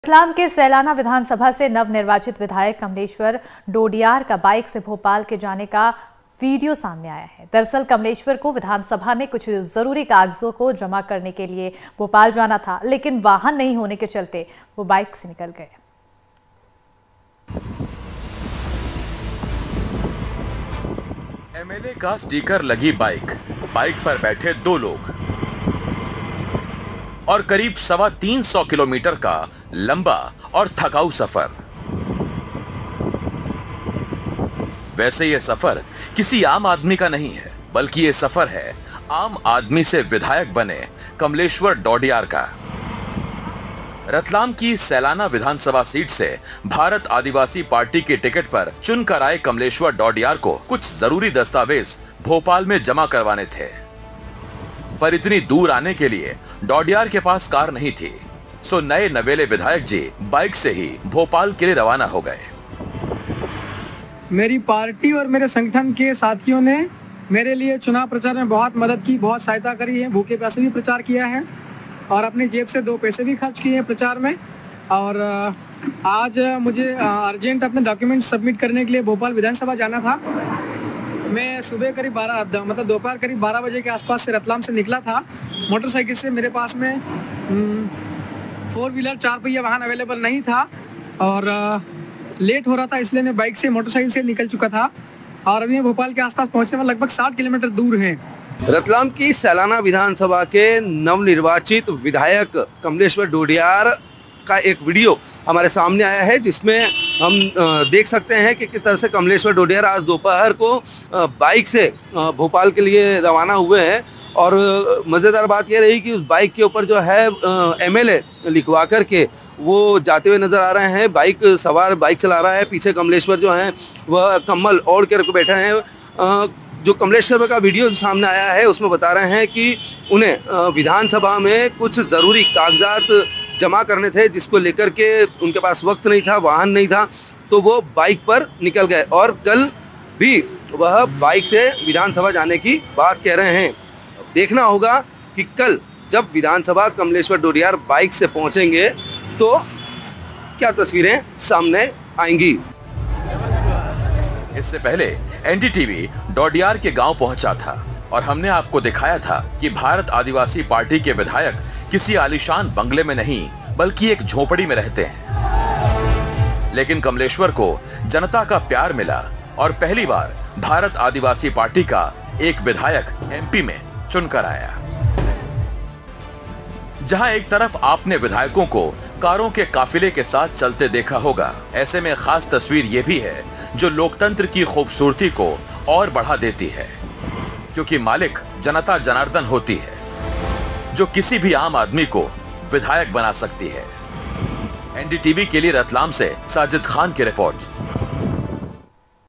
MP Election Results: वाहन नही मिला तो Bike से ही निकल पड़े MLA Kamaleshwar Dodiyar, Video Viral
MP Election Results: रतलाम (Ratlam) से सैलाना विधानसभा (Sailana Assembly) से नवनिर्वाचित विधायक (Newly Elected MLA) कमलैश्वर डोडियार (Kamaleshwar Dodiyar) का बाइक (Bike) से भोपाल (Bhopal) जाने का वीडियो तेजी से सोशल मीडिया पर वायरल (Viral) हो रहा है.